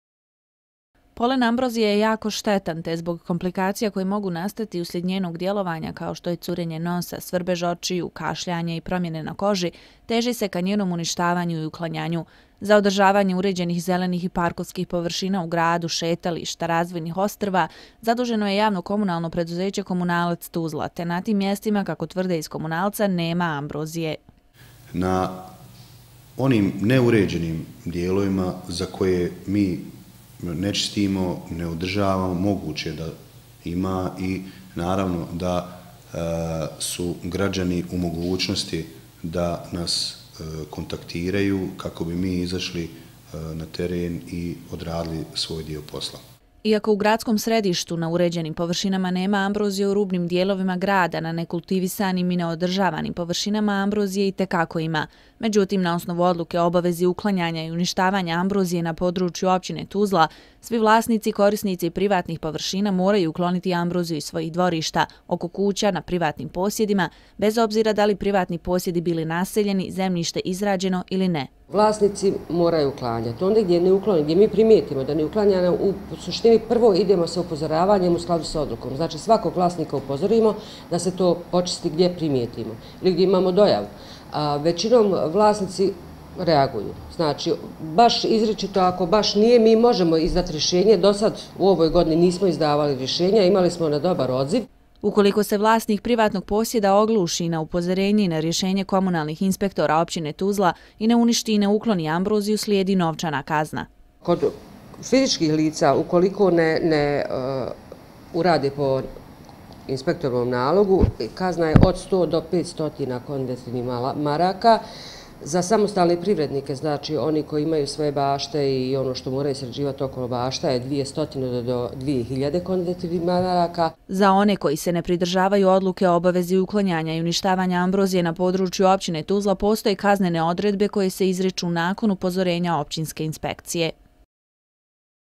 Ambrozija – alergijske reakcije i uklanjanje -12.08.2014.
Vrlo rijetko se za biljku može reći da je neprijatelj čovjekovog organizma, no ambrozija to 100 posto jeste. S tim u vezi uposlenici JKP „Komunalac“ Tuzla kontinuirano, u okviru redovnih aktivnosti rade na uništavanju ambrozije, no odgovornost za uklanjanje ambrozije  trebaju preuzeti i građani.

Autorska prava i distribucija:
AdRev & RTV SLON